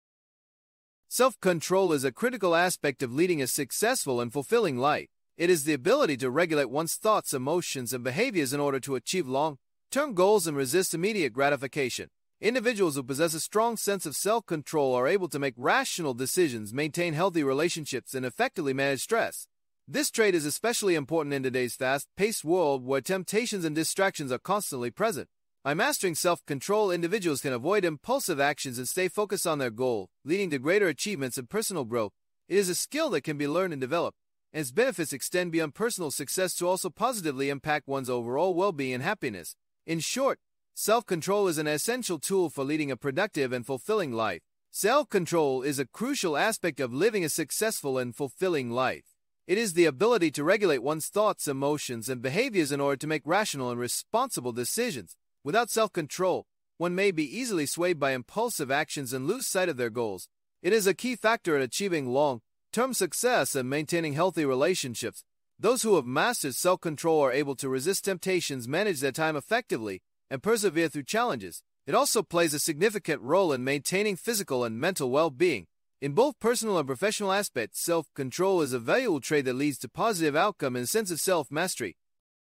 The Importance of Self-Control
What is the importance of self-control? Is self-control one of your strongest qualities?
Self-control is a critical aspect of leading a successful and fulfilling life. It is the ability to regulate one's thoughts, emotions, and behaviors in order to achieve long-term goals and resist immediate gratification. Individuals who possess a strong sense of self-control are able to make rational decisions, maintain healthy relationships, and effectively manage stress. This trait is especially important in today's fast-paced world, where temptations and distractions are constantly present. By mastering self-control, individuals can avoid impulsive actions and stay focused on their goals, leading to greater achievements and personal growth. It is a skill that can be learned and developed, and its benefits extend beyond personal success to also positively impact one's overall well-being and happiness. In short, self-control is an essential tool for leading a productive and fulfilling life.